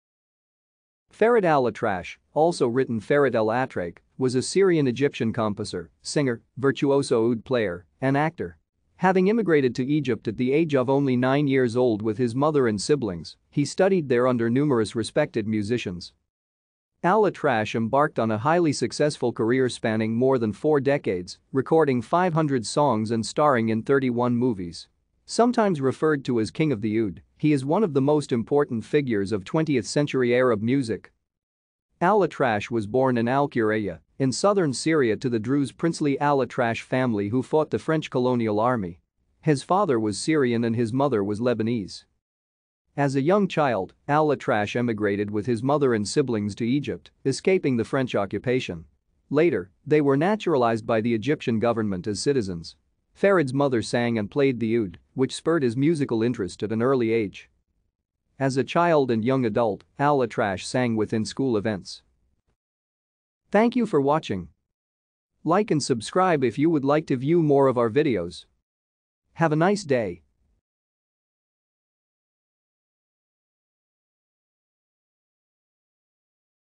Farid al-Atrash
Farid al-Atrash, also written Farid El-Atrache, was a Syrian-Egyptian composer, singer, virtuoso oud player, and actor. Having immigrated to Egypt at the age of only nine years old with his mother and siblings, he studied there under numerous respected musicians.